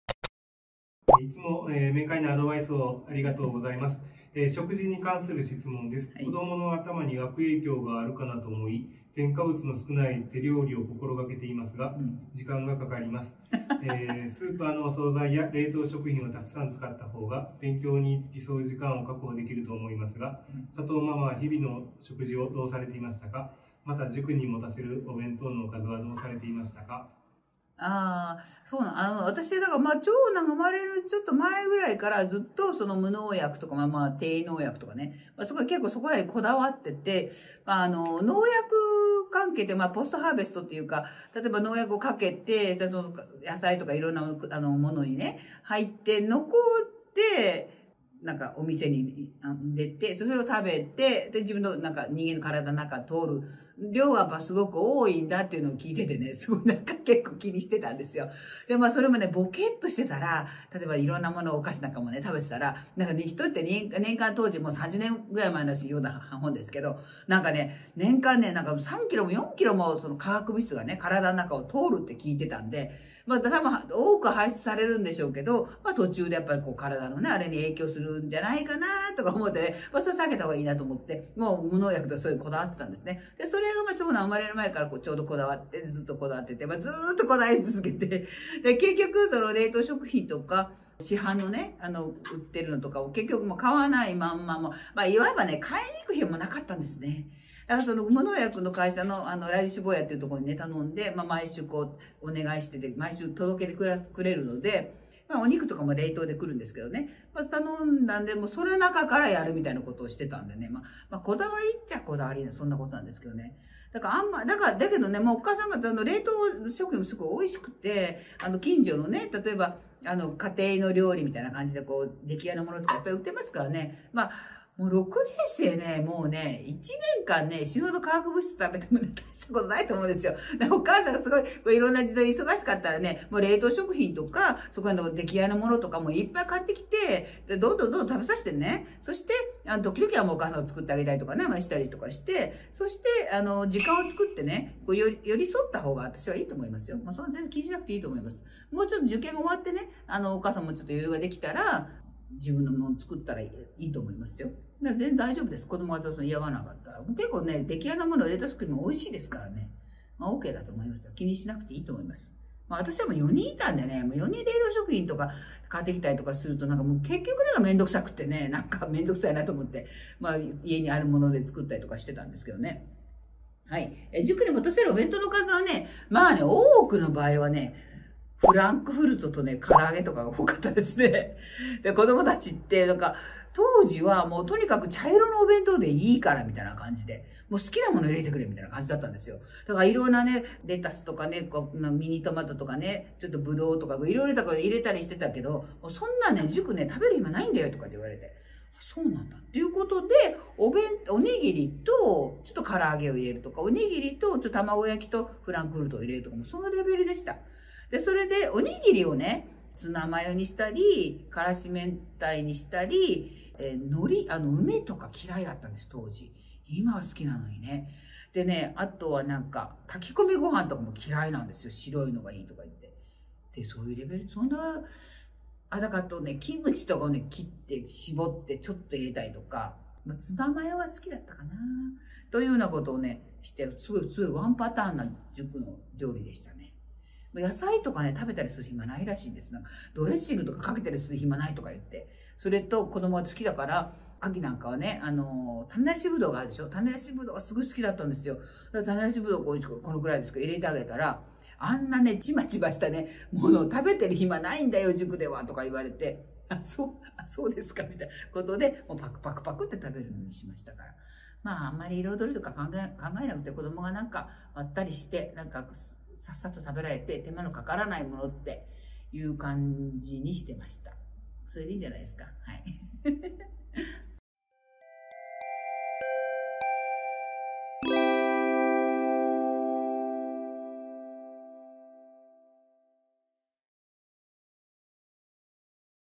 【佐藤ママが語る！】「日々の食事や塾弁」
灘中へ３人合格、お嬢様は洛南中に合格され、そして東大理Ⅲへ全員進学された佐藤ママこと佐藤亮子（浜学園アドバイザー）さんのノウハウを紹介していきます。

今回は次の質問にご回答いただきました。

いつも明快なアドバイスをありがとうございます。
食事に関する質問です。
子どもの頭に悪影響があるかなと思い
添加物の少ない手料理を
心がけていますが時間がかかります。
スーパーの総菜や冷凍食品をたくさん使った方が
勉強に付き添う時間を確保できると思いますが
佐藤ママは日々の食事をどうされていましたか？
また塾に持たせるお弁当のおかずは
どうされていましたか？

【企画・編集】
浜学園

【毎月第２日曜実施の浜学園公開学力テスト】